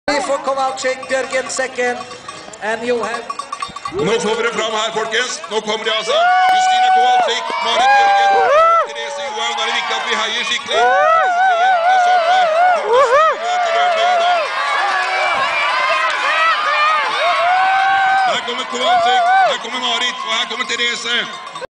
Holmenkollen Day 2012-1
Marit Bjørgen, Justyna Kowalczyk and Therse Johaug speeding past on their first lap of the women's 30km race!!